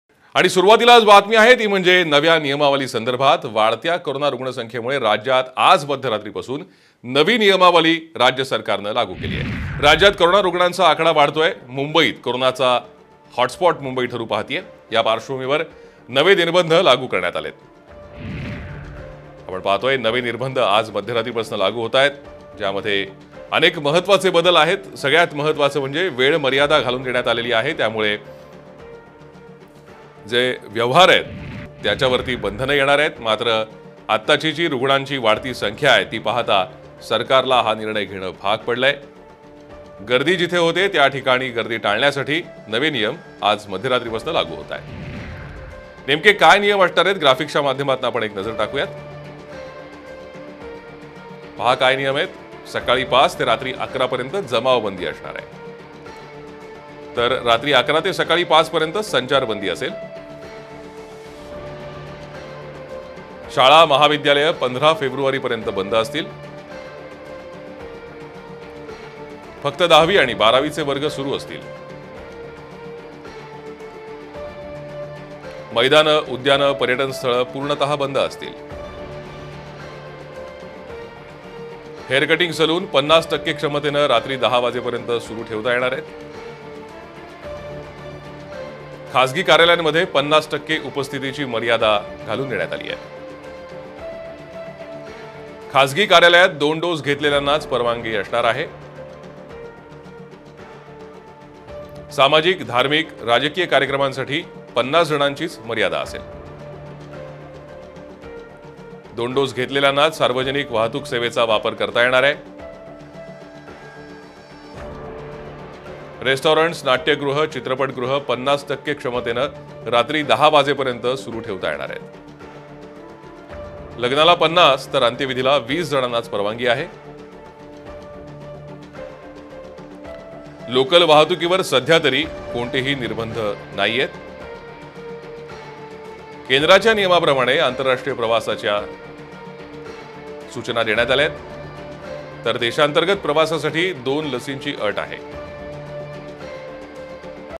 Covid New restrictions apply from midnight | राज्यात मध्यरात्रीपासून नवे निर्बंध लागू | Sarkarnama
सकाळी 5 ते 11 रात्री 11 पर्यंत जमावबंदी. रात्री 11 ते सकाळी 5 संचारबंदी. शाळा महाविद्यालयं 15 फेब्रुवारीपर्यंत बंद. लग्नसोहळ्याला 50 टक्के परवानगी.